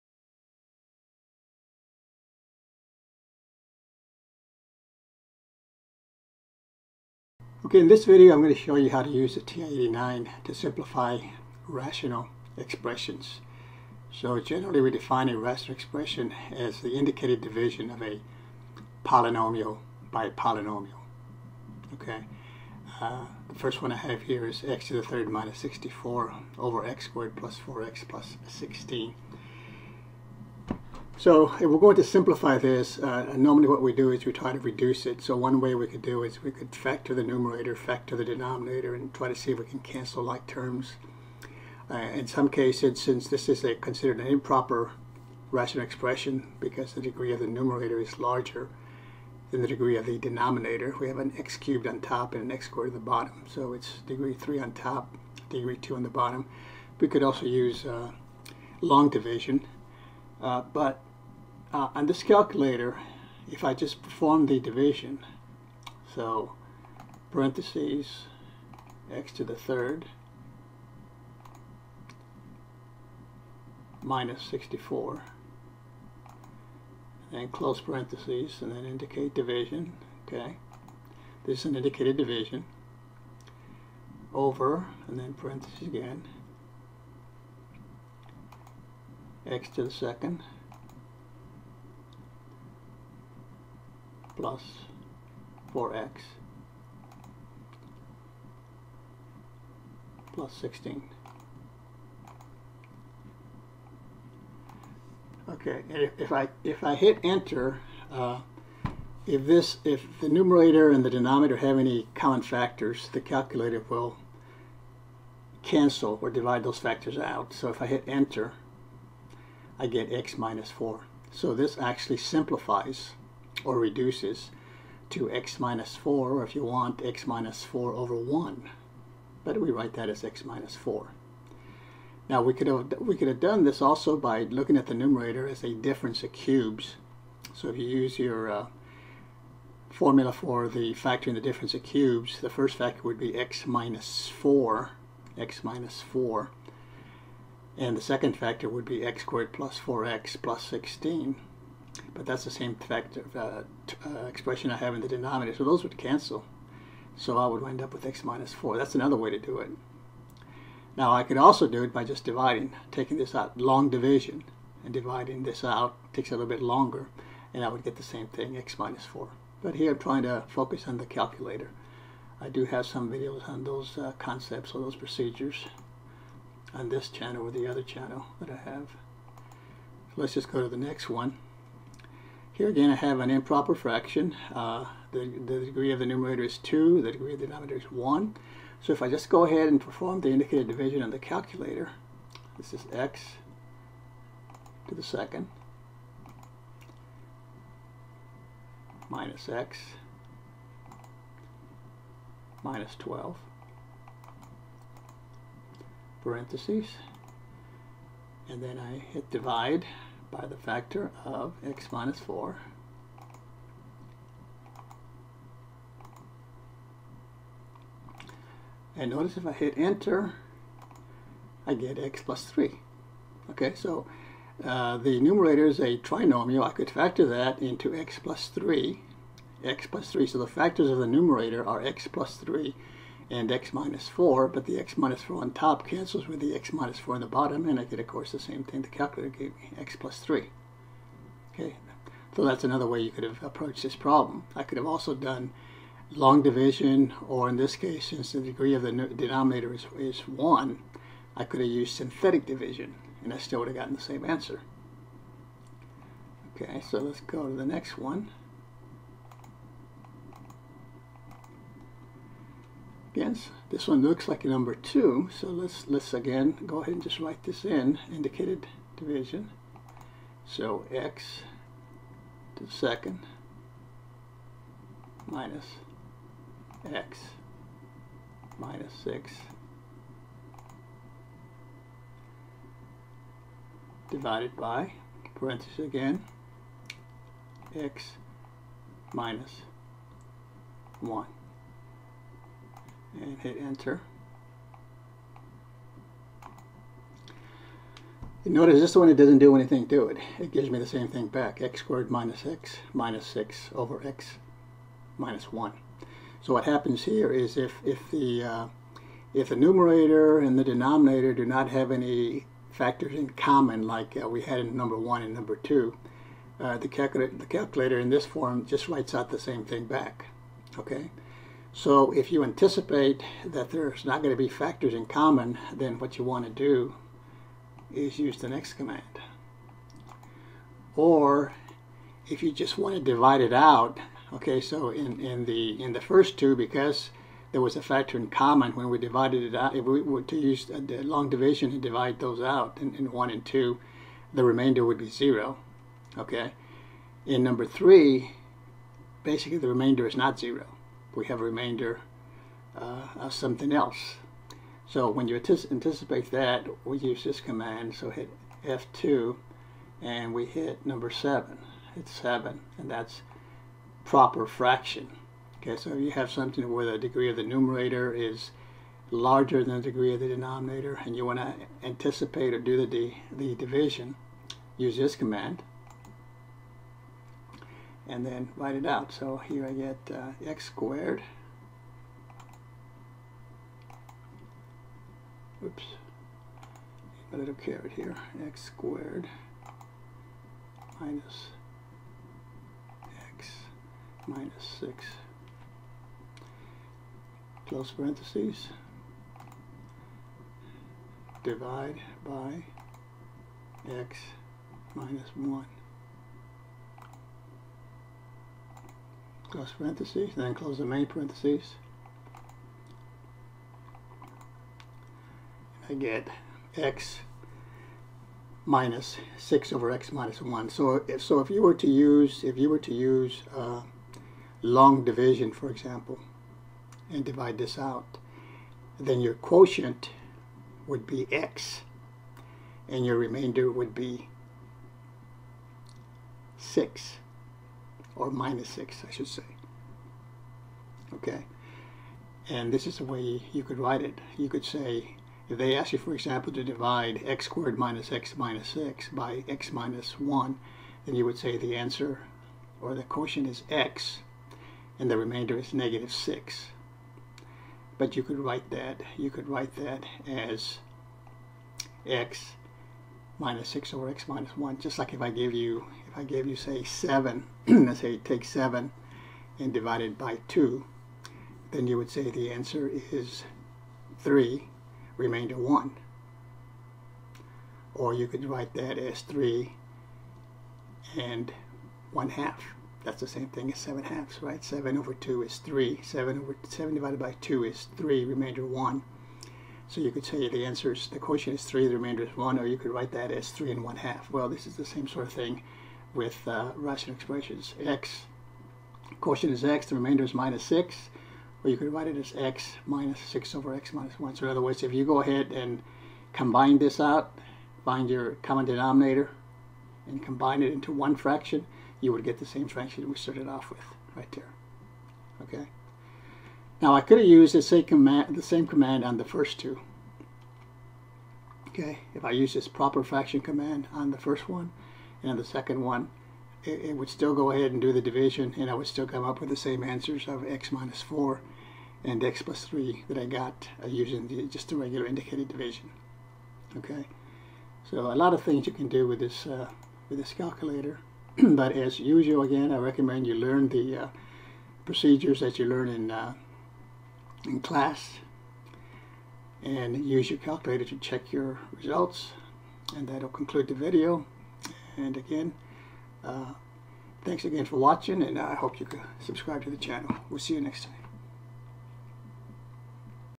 Simplify Rational Expressions With The TI-89
In this video tutorial, we use the TI-89 graphing calculator to simplify rational expressions.